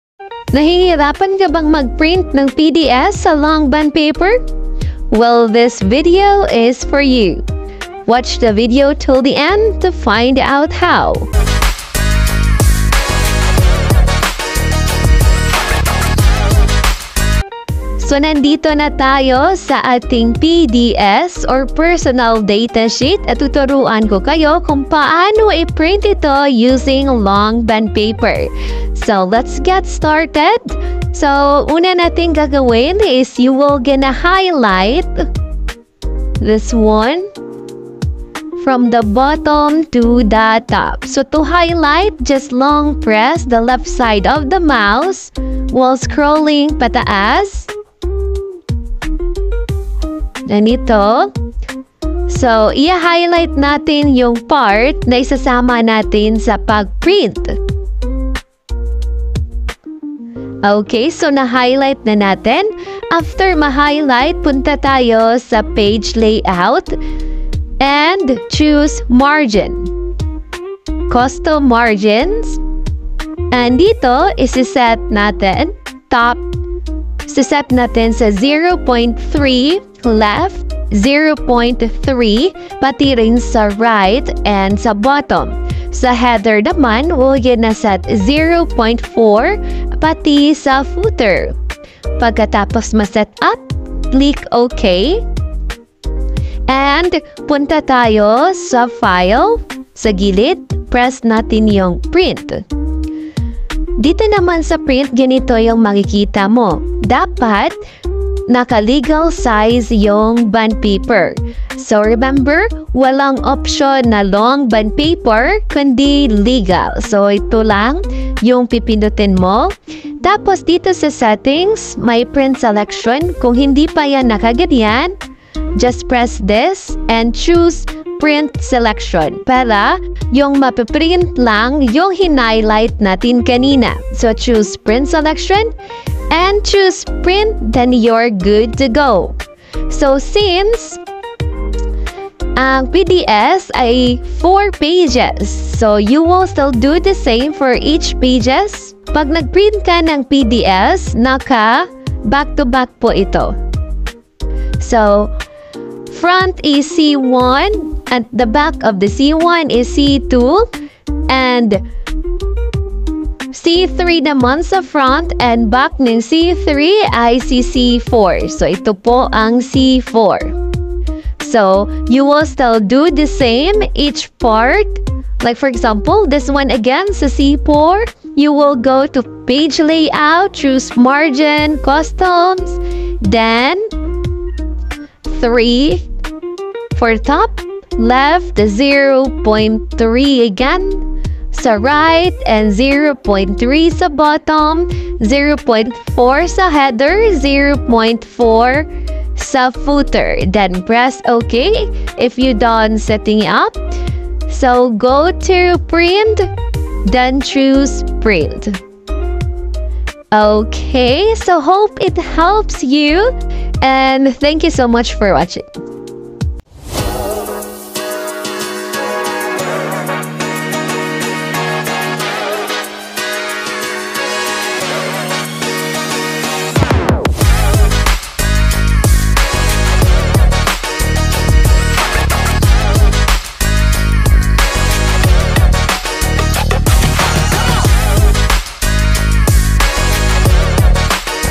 Paano magprint ng PDS (Personal Data Sheet) sa long bond paper using Excel? | How to print PDS?
Easy steps sa pagprint ng PDS sa long bond paper using Excel. | How to print PDS in a long bond paper?